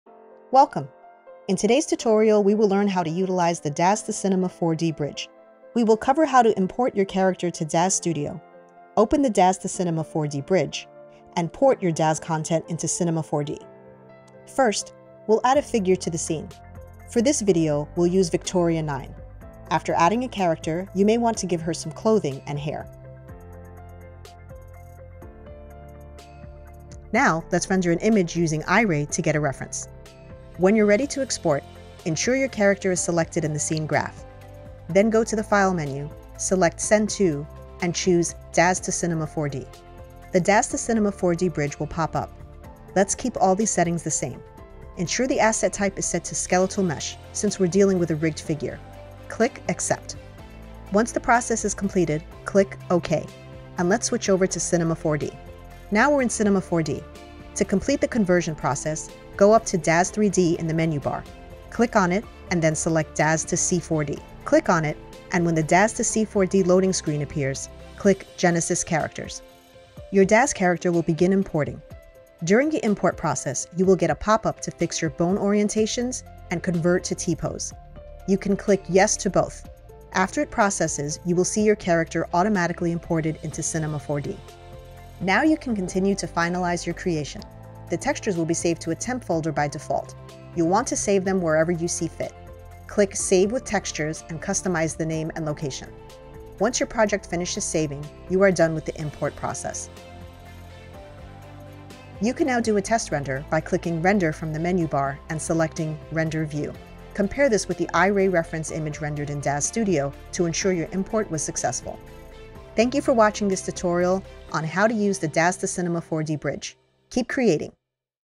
Daz3D to Cinema 4D Bridge: Basic Usage Tutorial
Discover the power of the Daz to Cinema 4D Bridge as we delve into the process of importing Daz Studio characters into Cinema 4D. This tutorial covers everything from character selection and customization to exporting and rendering.  Whether you're a beginner or an experienced artist, this tutorial will guide you through the essential steps.

Learn how to optimize bone orientations, convert to T-pose, and save textures in your preferred location.  Watch now and start bringing your Daz Studio characters to life in Cinema 4D!